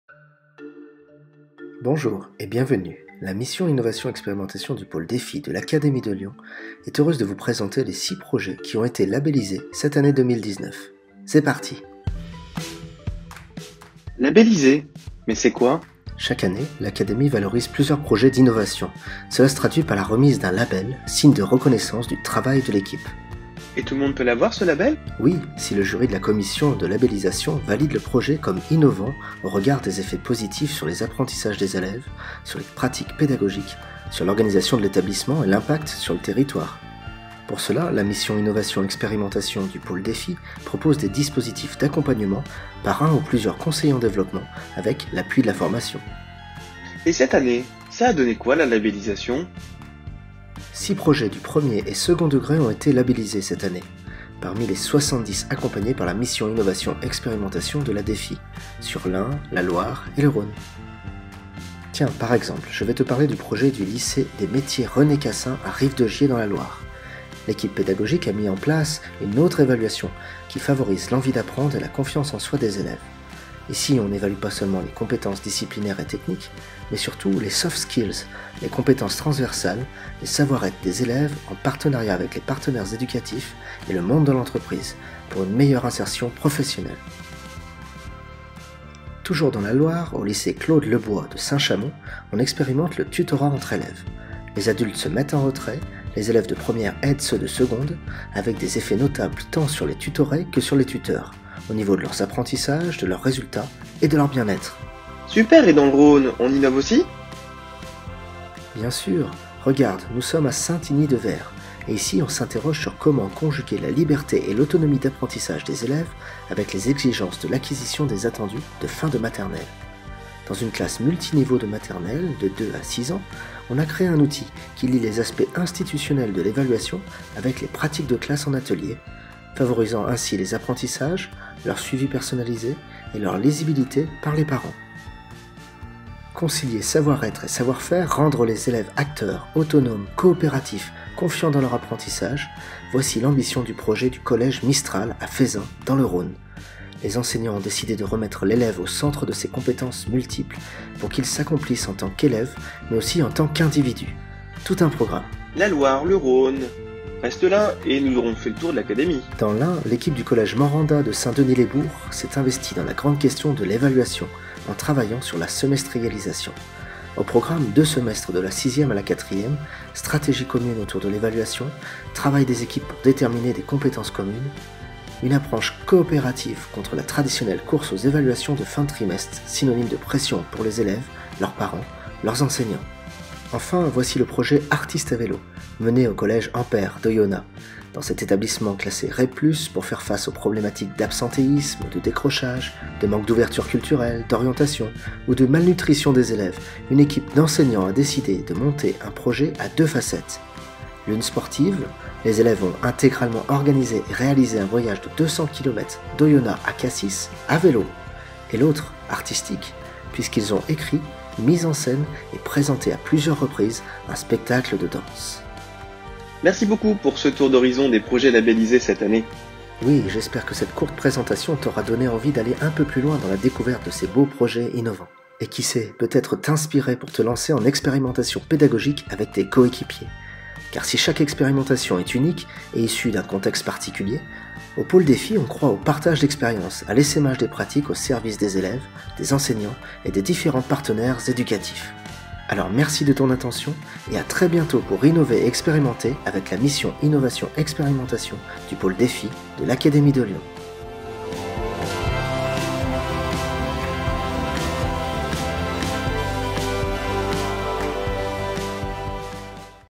Présentation des projets labellisés 2019
La Mission Innovation Expérimentation du Pôle Dfie de l'Académie de Lyon labellise chaque année des projets innovants. Cette année, ce sont 6 projets qui ont été retenus sur les 70 accompagnés. En voici une courte présentation !